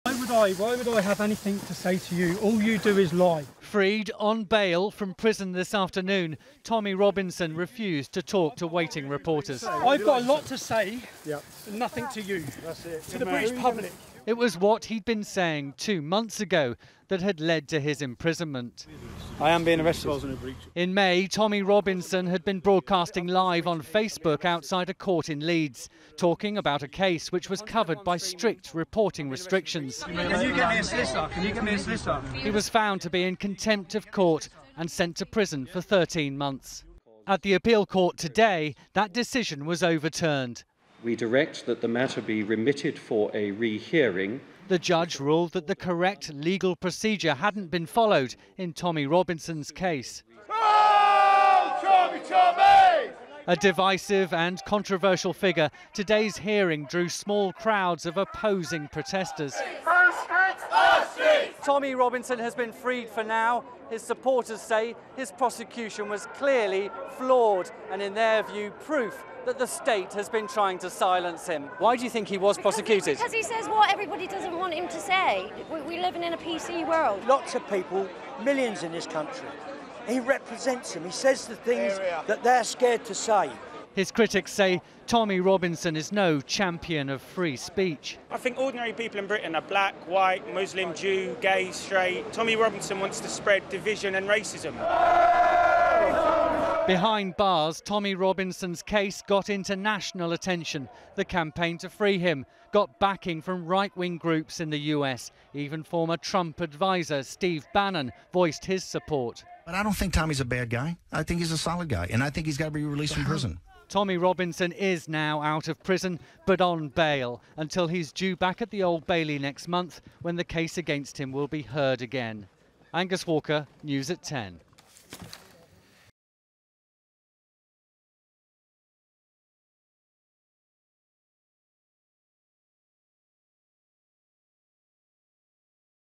Tommy Robinson freed on bail after winning contempt challenge | ITV News
Former English Defence League leader Tommy Robinson has been freed on bail by leading judges after winning his challenge against a contempt of court finding.

The right-wing campaigner refused to answer questions from journalists as he was led by supporters from HMP Onley in Rugby but told members of the media "you've lost the faith of the British public".

Three judges in London quashed a finding made against Robinson at Leeds Crown Court in May where he was sentenced to 13 months in jail.

The contempt matter will be heard again at court at a future date.